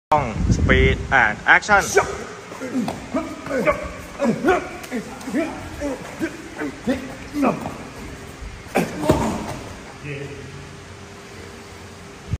Mew Suppasit trains for Mr Hiiragi thai remake
Thailand is doing a remake for me hiiragi, a Japanese 2019 drama, and mew Suppasit will play the teacher.